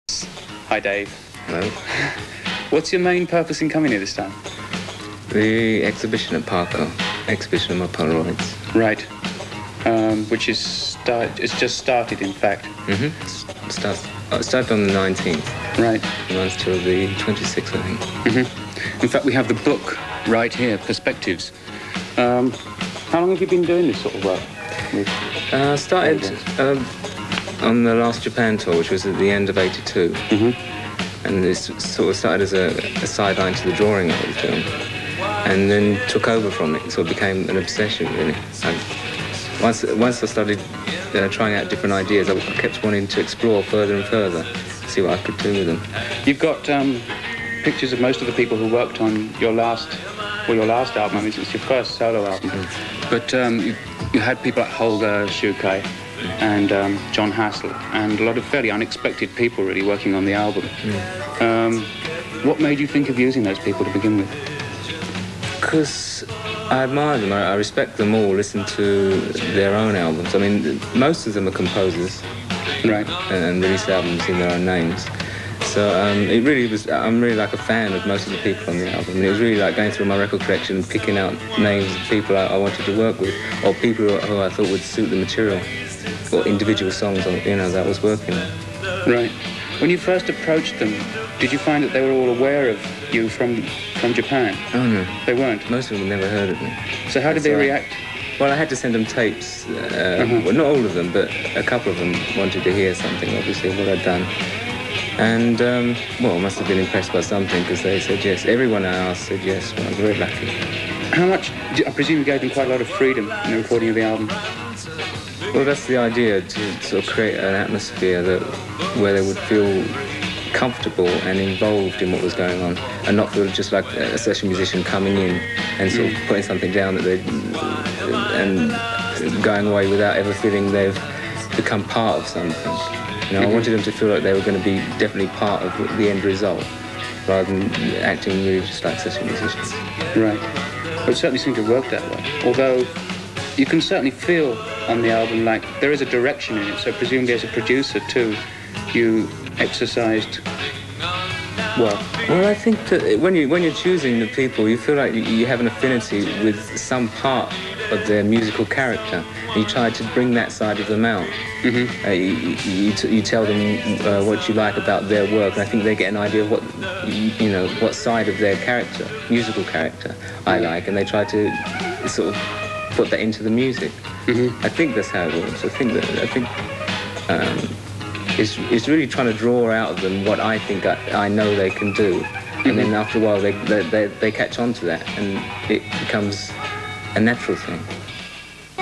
David Sylvian - Interview On Japanese TV
Interview on Japanese TV with Peter Barakan, around the time of his Polaroid exhibition.